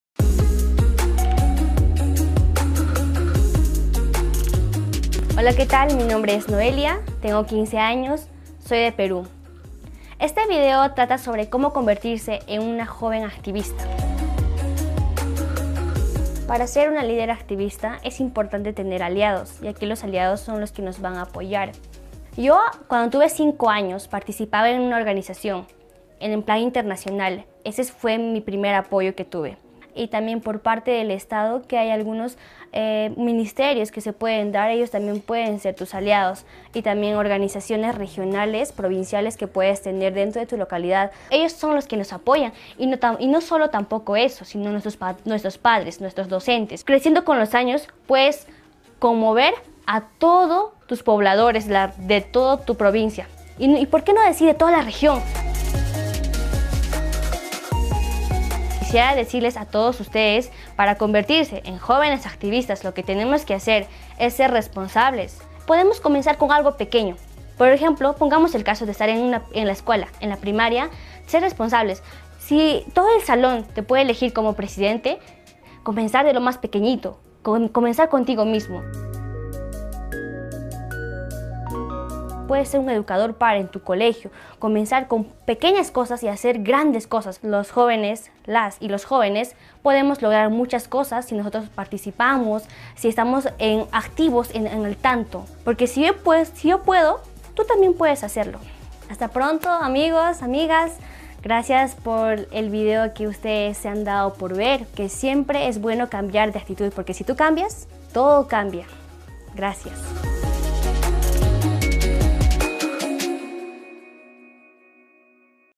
Zo word je een jongerenactivist! | Plan International Nederland
Hoe word je een jongerenactivist? De vijftienjarige Nohelia uit Peru legt het je uit! Zelf strijdt ze al van jongs af aan voor gelijke rechten voor meisjes en jongens. Ze was zes jaar lang klassenvertegenwoordiger en is sindsdien nog steeds actief op haar school. Zo komt ze op voor de rechten van meisjes en brengt onderwerpen als tienerzwangerschappen onder de aandacht.